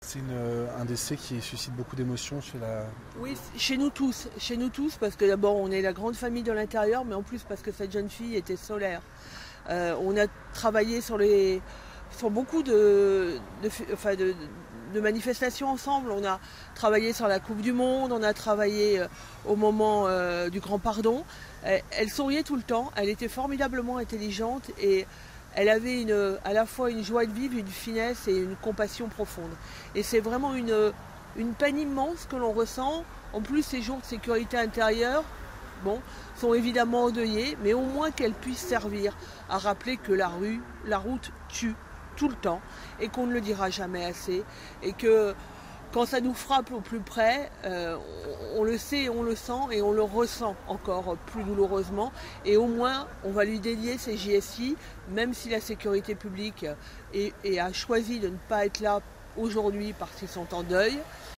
Françoise Souliman, Préfète de la Haute-Marne
Mme Françoise Souliman, Préfète de la Haute-Marne, réagit au décès accidentel de l'adjointe de sécurité de la Police Nationale, lors d'un accident de la route, ce samedi 13 octobre.